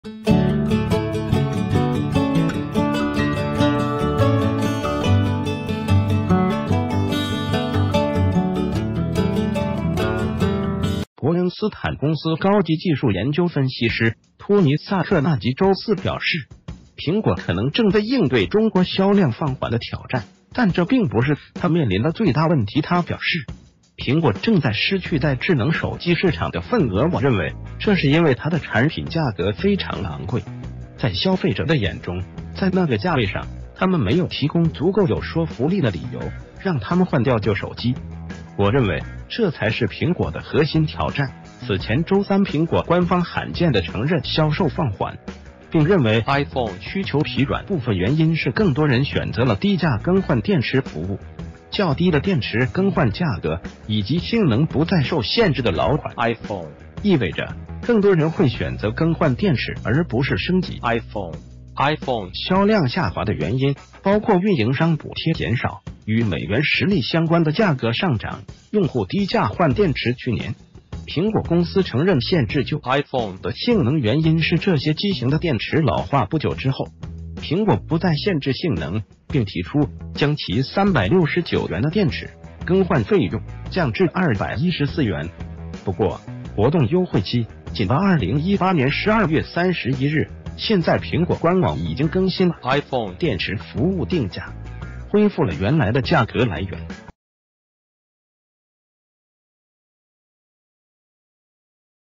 蘋果稱低價換電池導致銷量下滑，分析師：你賣太貴
谢谢！

    伯恩斯坦公司高級技術研究分析師托尼·薩克納吉周四表示，蘋果可能正在應對中國銷量放緩的挑戰，但這并不是它面臨的最大問題。他表示，“蘋果正在失去在智能手機市場的份額。我認為，這是因為它的產品價格非常昂貴，在消費者的眼中，在那個價位上，它們沒有提供足夠有說服力的理由讓他們換掉舊手機。我認為，這才是蘋果的核心挑戰。”此前周三，蘋果官方罕見地承認銷售放緩，并認為iPhone需求疲軟，部分原因是更多人選擇了低價更換電池服務。較低的電池更換價格，以及性能不再受限制的老款iPhone，意味著更多人會選擇更換電池，而不是升級iPhone。iPhone銷量下滑的原因包括運營商補貼減少、與美元實力相關的價格上漲、用戶低價換電池去年，蘋果公司承認限制舊iPhone的性能，原因是這些機型的電池老化。不久之后，蘋果不再限制性能，并提出將其369元的電池更換費用降至214元。不過活動優惠期僅到2018年12月31日，現在蘋果官網已經更新了iPhone電池服務定價，恢復了原來的價格。
    來源：鳳凰科技